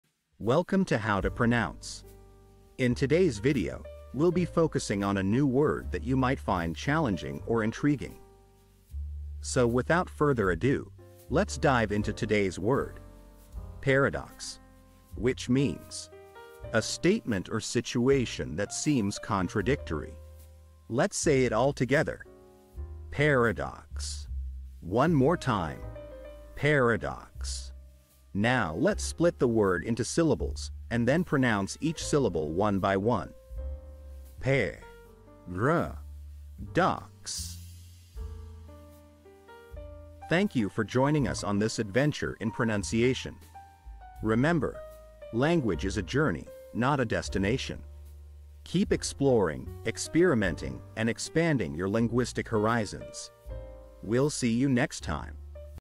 How to pronounce Paradox
🌍 Learn a new language now Master the pronunciation of 'Paradox' Paradox 📖 - which means : A statement or situation that seems contradictory 📝 - with this quick and clear tutorial by How To Pronounce!

In this video 📽️, we'll guide you through the correct pronunciation of 'Paradox' in English, using native speaker examples 🗣️👂 and easy-to-follow phonetic breakdowns 🎵. Whether you're learning English for travel ✈️, business 💼, or just for fun 🎉, our guide will help you perfect your pronunciation in no time ⏰.